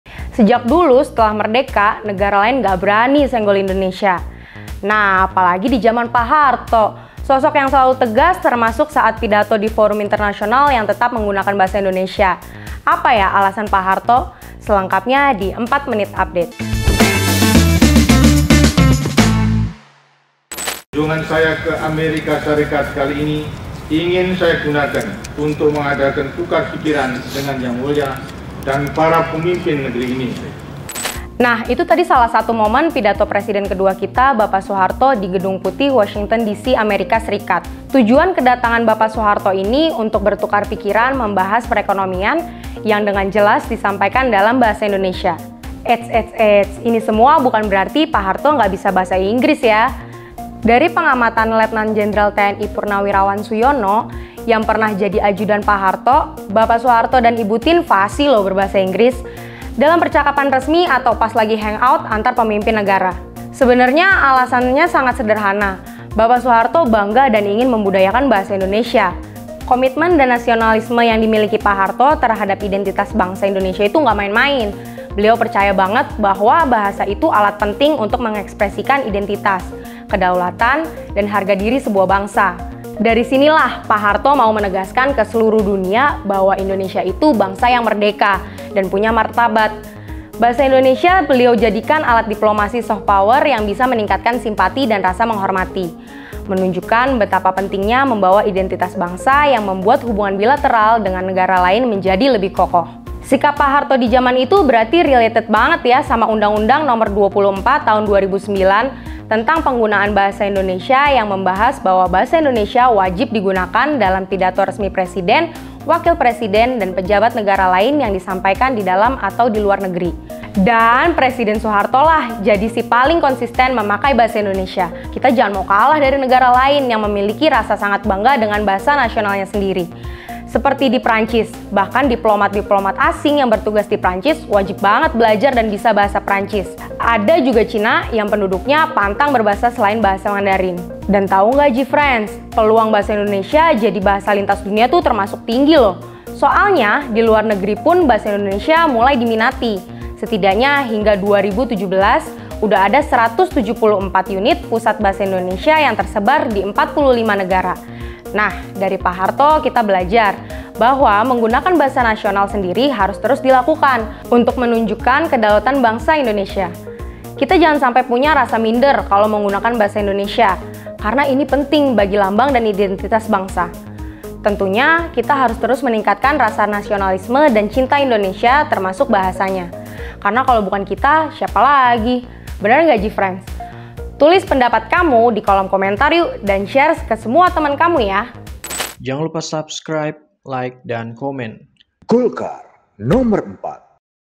4 MU - TERUNGKAP! ALASAN SOEHARTO PAKAI BAHASA INDONESIA
Meskipun bisa Bahasa Inggris, nggak jadi alasan buat terus bangga menggunakan bahasa nasional sendiri. Seperti Soeharto yang selalu pakai bahasa Indonesia setiap pidato atau kunjungan internasional loh. Wah keren!
Tapi apa ya alasan sebenarnya Presiden Kedua RI ini melakukan itu?